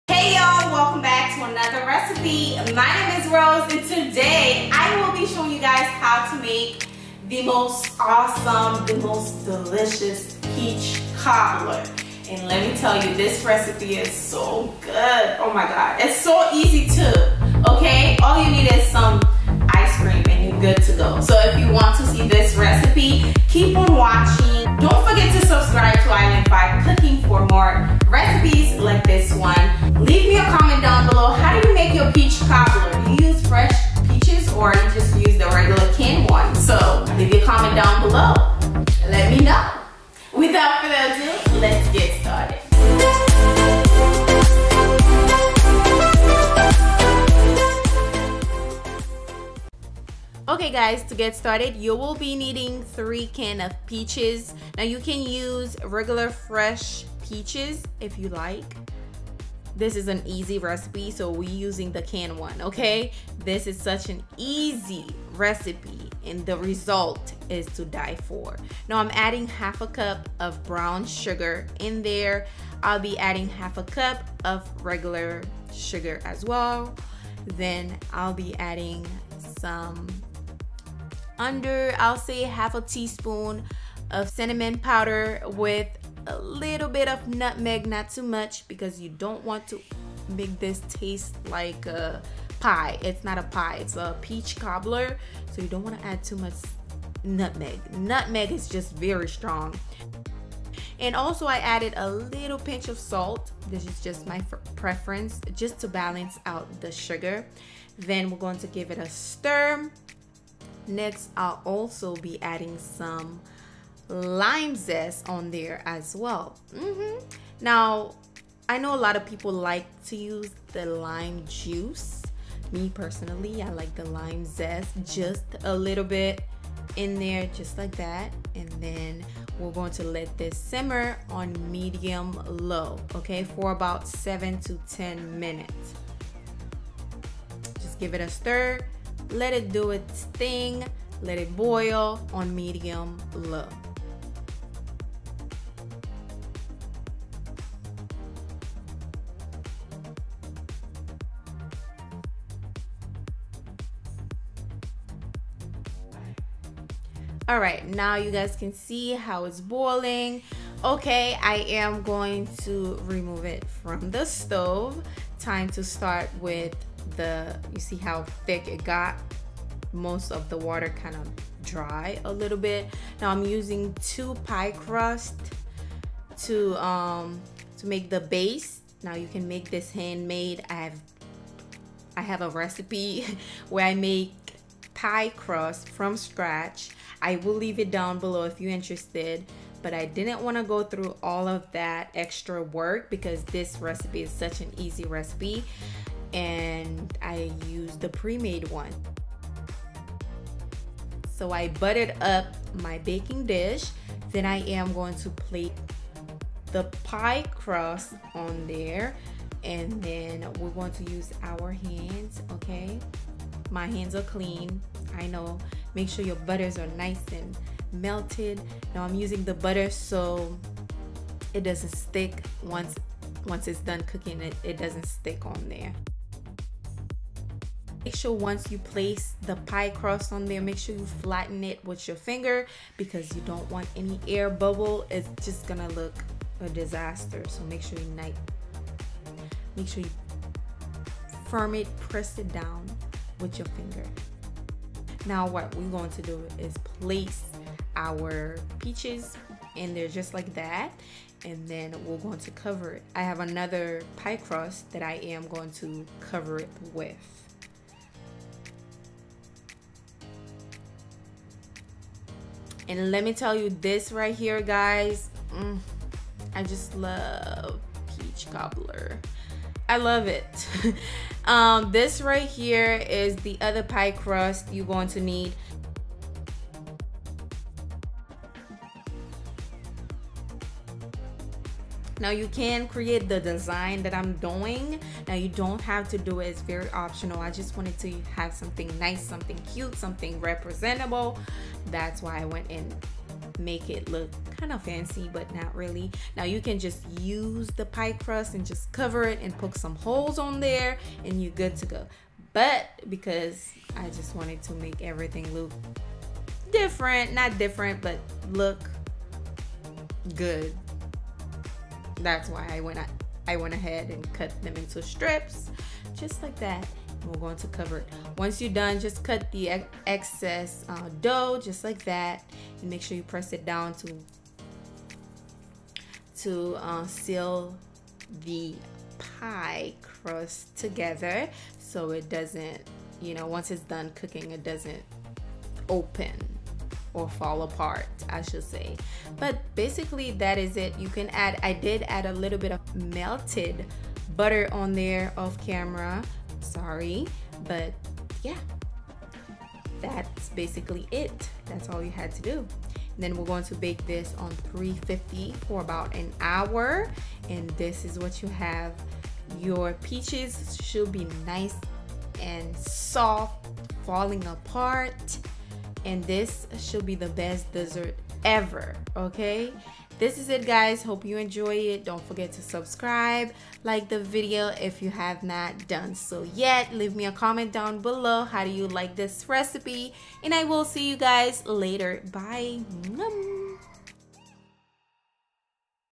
Peach Cobbler | Peach Cobbler Recipe
This Peach Cobbler is so amazing!

INGREDIENTS!
3 can peaches
1/2 cup brown sugar
1/2 granulated sugar
1/2 cinnamon
1/2 vanilla extract
1/4 tsp nutmeg
a pinch of salt
lime zest
1 pie crust

Bake on 350 for 50 minutes. at the lower rack of your oven
My Pots and Pan

Most Popular Recipes

Follow my personal social media accounts

Located in South Florida